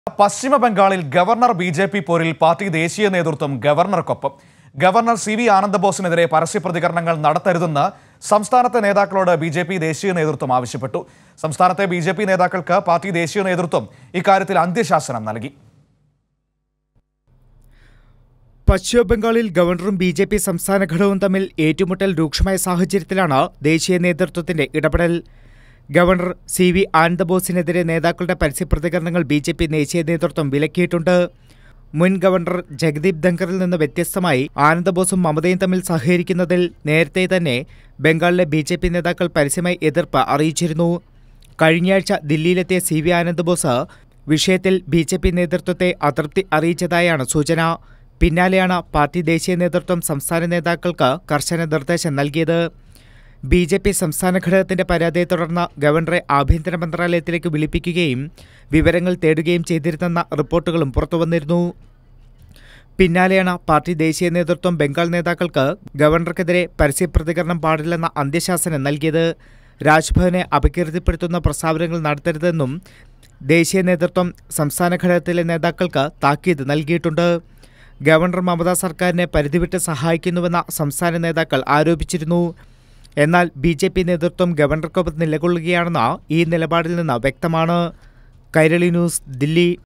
പശ്ചിമബംഗാൾ ഗവർണ്ണർ- BJP പോരിൽ പാർട്ടി ദേശീയ നേതൃത്വം ഗവർണ്ണർക്കൊപ്പം
ഗവർണ്ണർ സി.വി. ആനന്ദ ബോസിനെതിരെ പരസ്യ പ്രതികരണങ്ങൾ നടത്തരുതെന്ന് സംസ്ഥാന നേതാക്കളോട് BJP ദേശീയ നേതൃത്വം ആവശ്യപ്പെട്ടു.പാർട്ടി ദേശീയ നേതൃത്വം ഇക്കാര്യത്തിൽ അന്ത്യശാസനം നൽകിയിട്ടുണ്ട്.